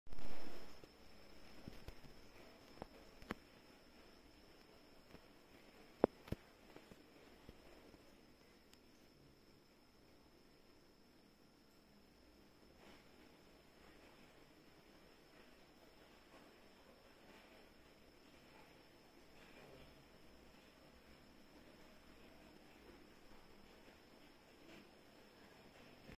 Why My Cat Drinking Water Like This??
My cat has a unique way to drinking water.